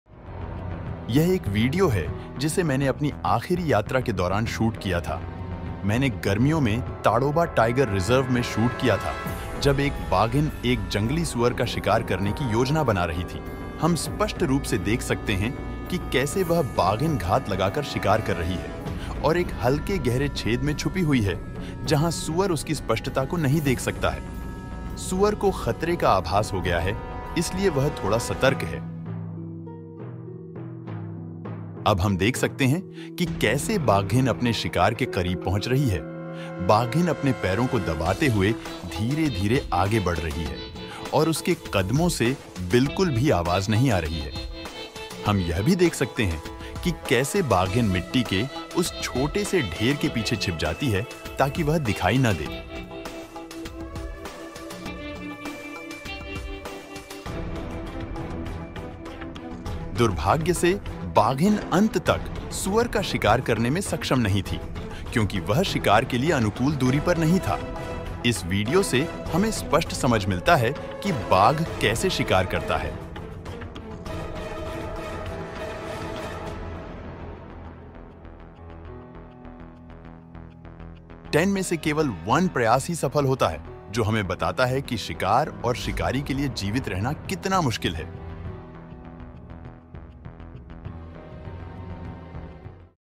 tiger attack 😱😨!!| tadoba tiger reserve| tadoba national park| wildlife photographer
This is a video from tadoba tiger reserve where the tigress madhu is attacking a wild boar.